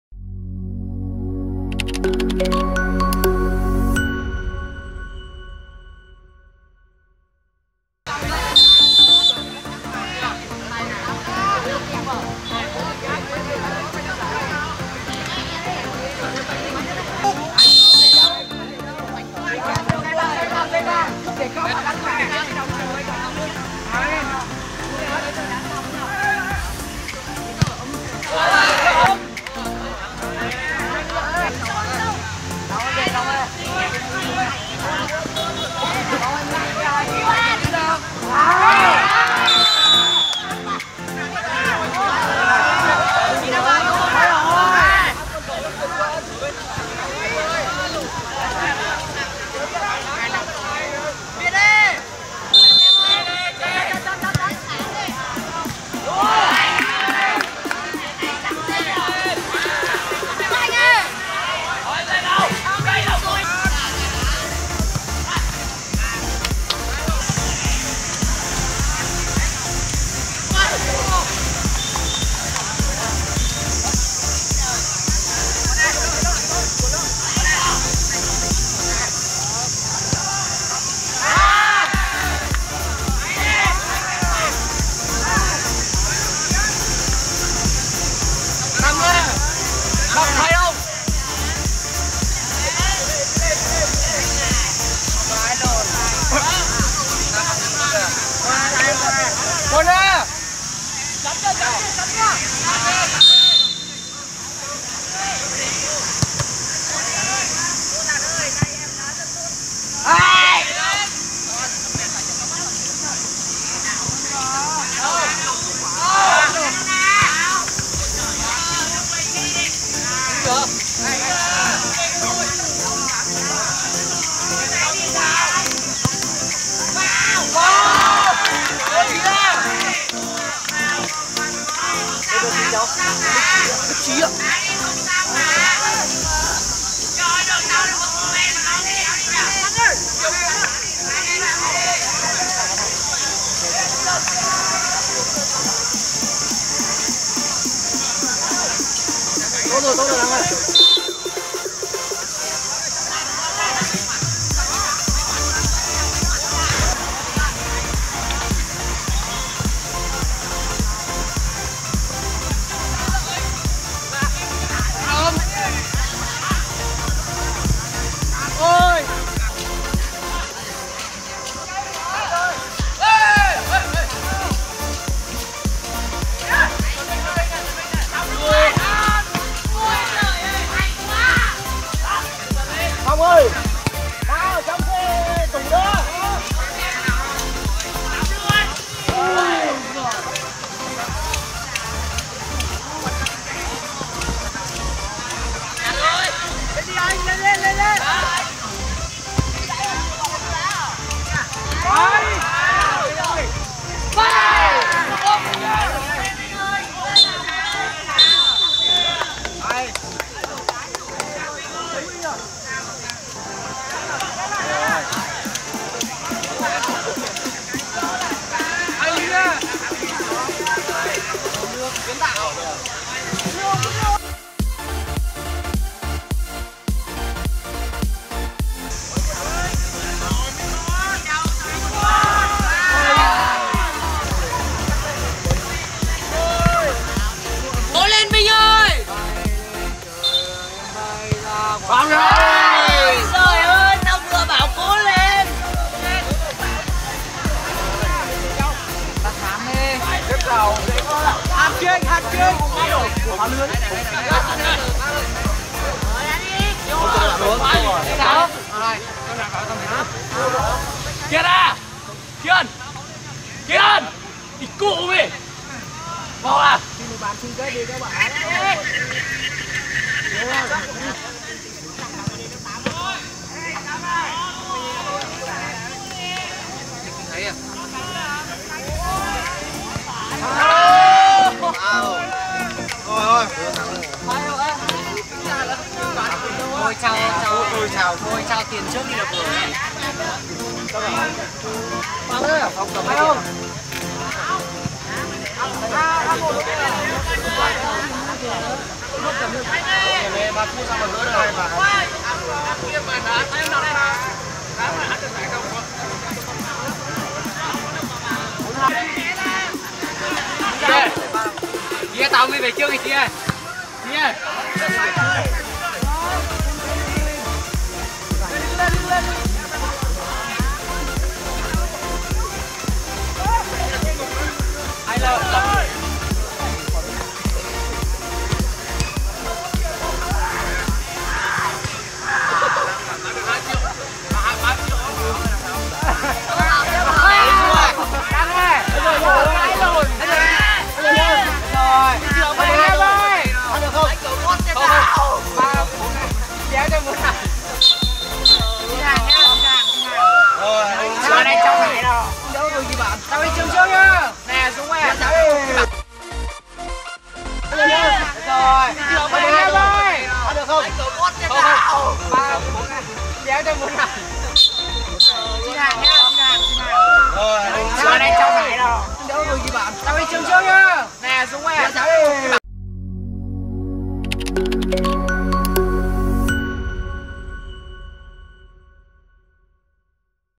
🎬 Highlights: LỚP 9A3 🆚 LỚP 9A1🏆 TRANH GIẢI 3 - GIẢI BÓNG ĐÁ THCS QUANG TRUNG - 2022
🎬 Highlights: LỚP 9A3 🆚 LỚP 9A1
🏆 TRANH GIẢI 3 - GIẢI BÓNG ĐÁ THCS QUANG TRUNG - 2022
⏰ 06h30
📅 Ngày 29/06/2022.
⛳ Sân Bóng Bình Minh - Tp Thanh Hóa